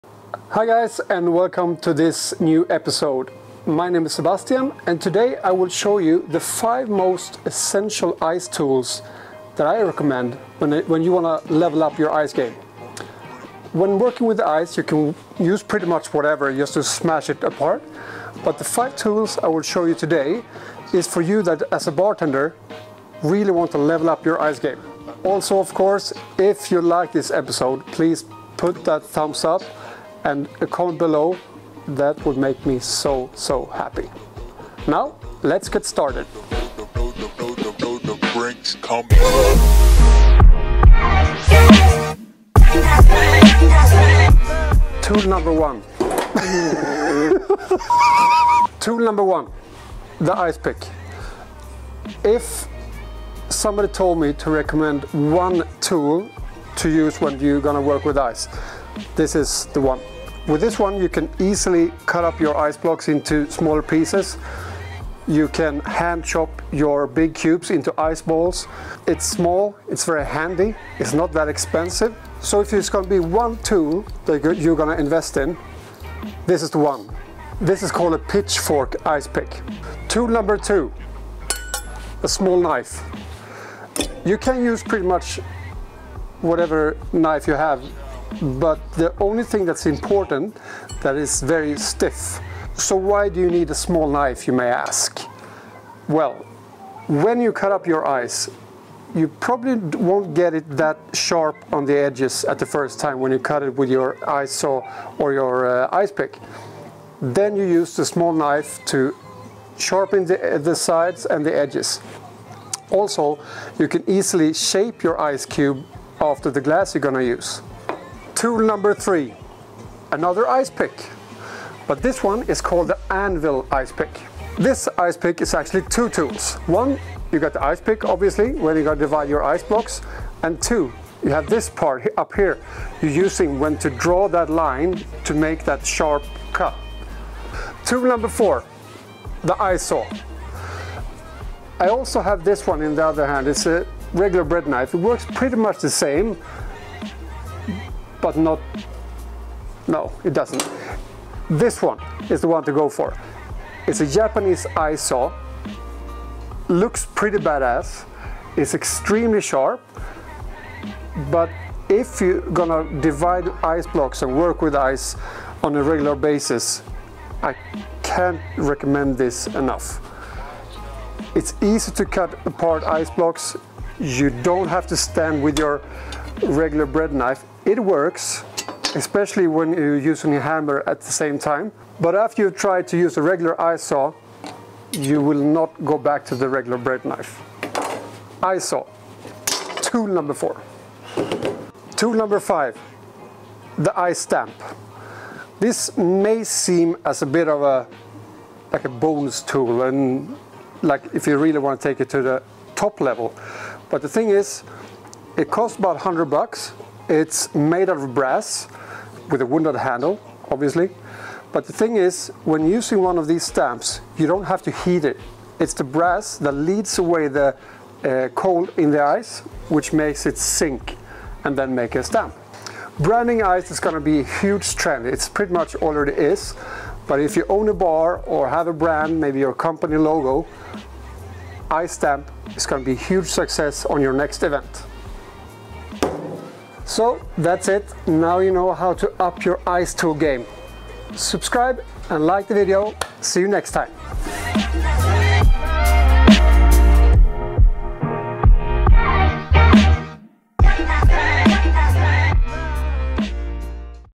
Top 5 ICE TOOLS To Level Up Your ICE GAME!
In this video, I'm going over my TOP 5 ICE TOOLS to level up your ice game! I'll show you my own go-to-tools and how you use them. All of these tools I use on a daily basis and I hope I'll catch your interest and get them for yourself!